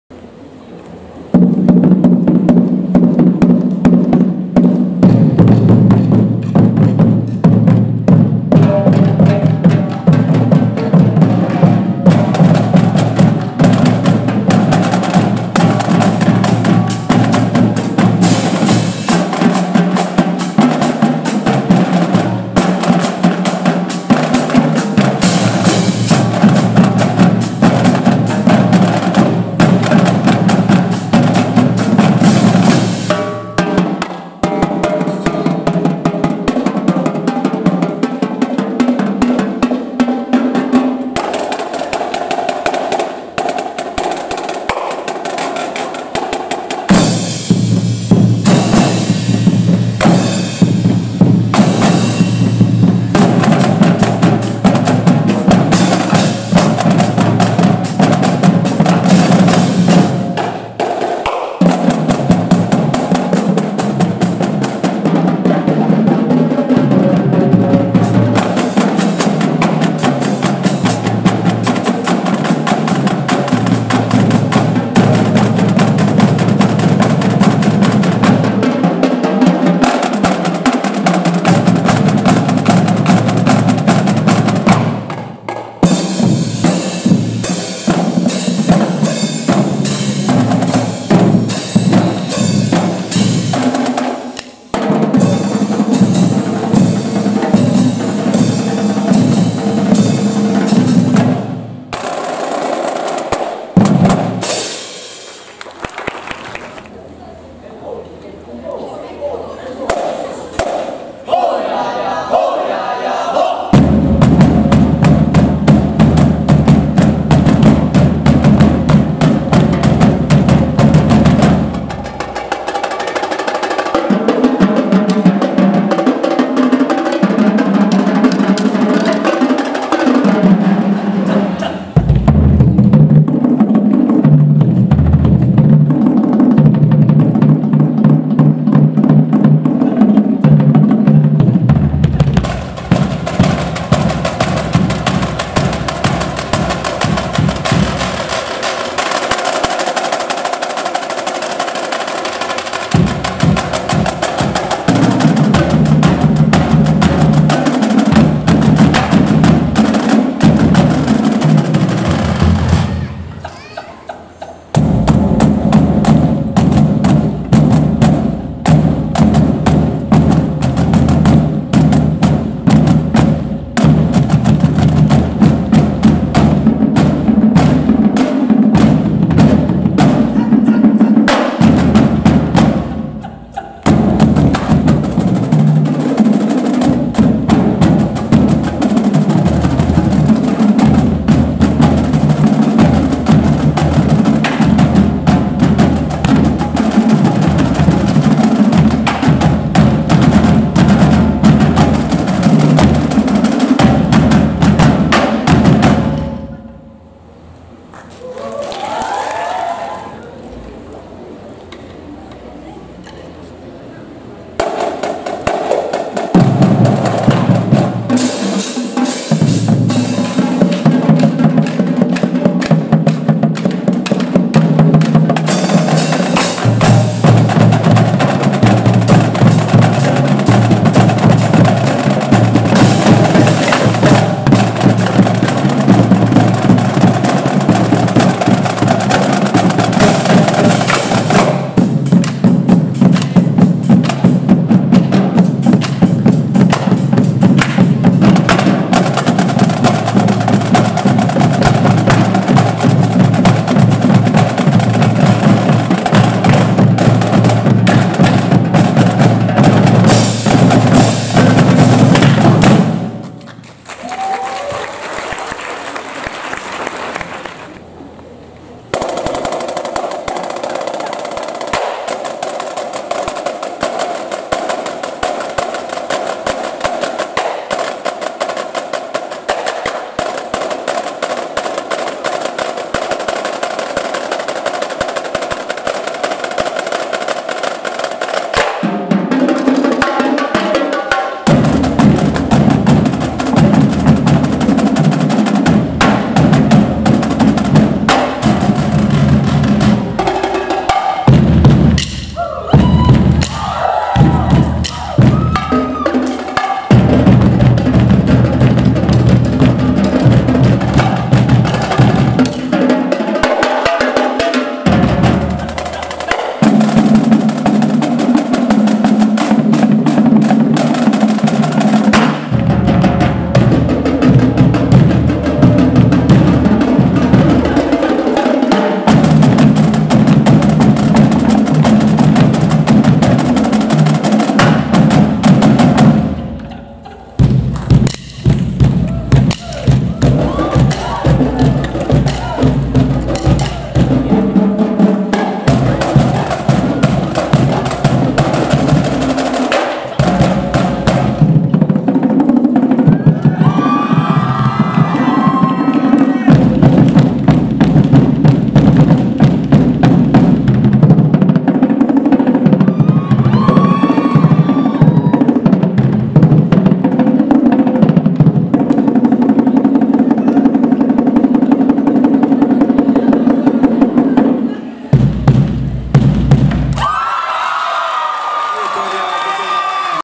Drumline Battle UiTM | Johor VS Perlis
Semi Final Drumline Battle UiTM

UiTM Johor : Green Phoenix Marching Band
UiTM Perlis : Frostbeat Regiment Percussion

Perlis won.